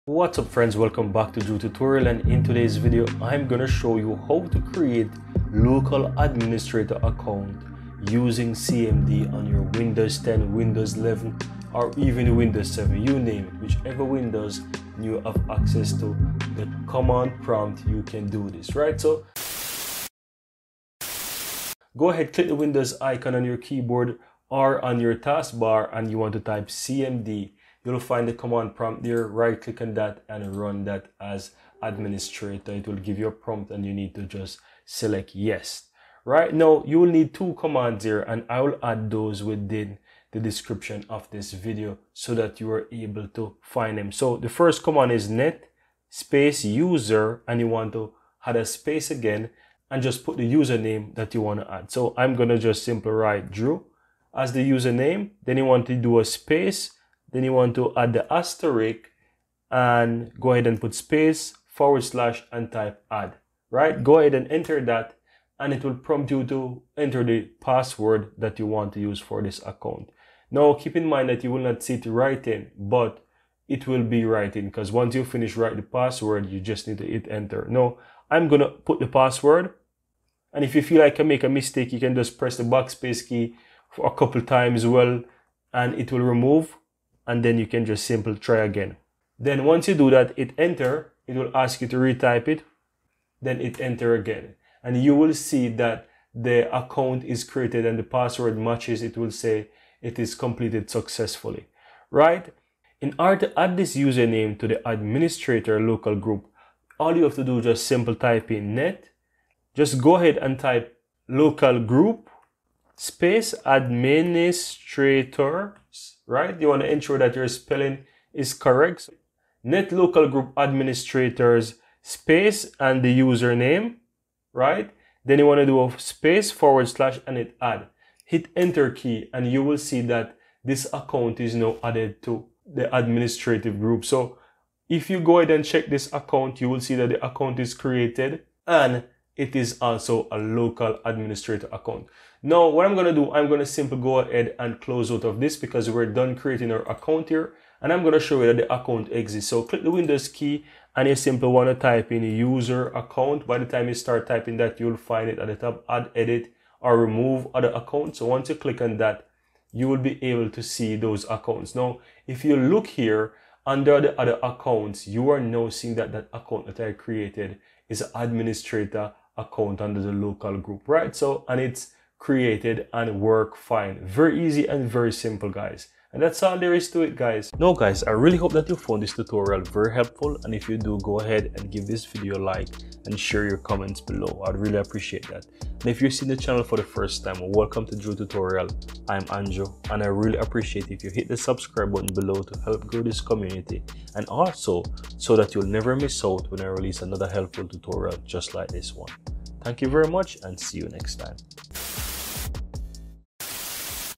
How to Create a Local Admin Account Through CMD in Windows 11/10
In this tutorial, I show you How to Create a Local Admin Account Through CMD in Windows 11/10.

Commands:
net user username * /add
net localgroup administrators username /add